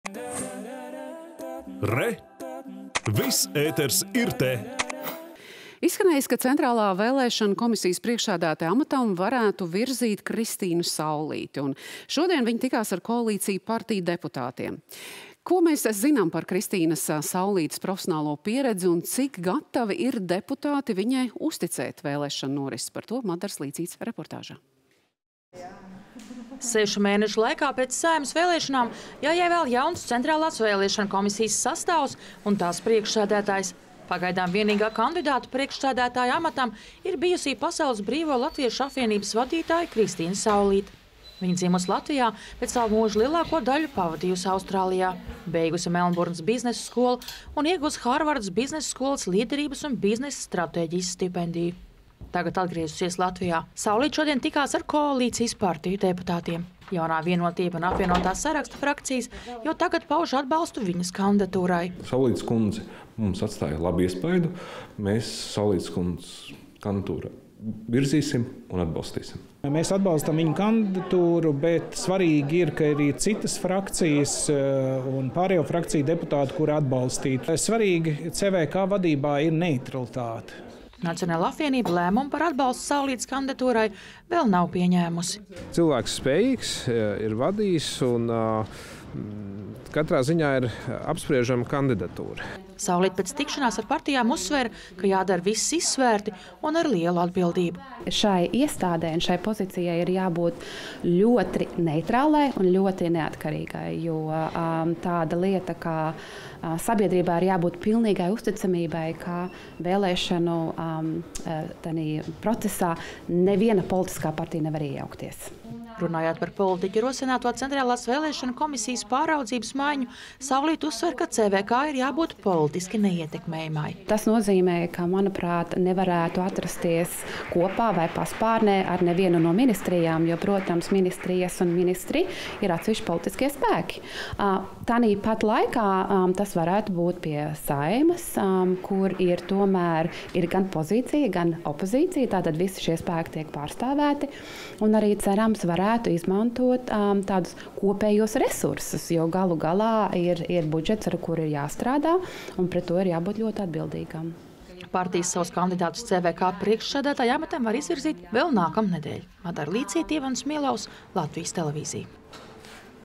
CVK priekšsēdētājas amata kandidāte – Kristīne Saulīte
Jau izskanējis, ka Centrālās vēlēšanu komisijas priekšsēdētājas amatam varētu virzīt Kristīni Saulīti. Šodien viņa tikās ar koalīcijas partiju deputātiem. Kas ir Kristīne Saulīte un cik gatavi ir deputāti viņai uzticēt vēlēšanu norisi – Madaras Līcītes reportāžā.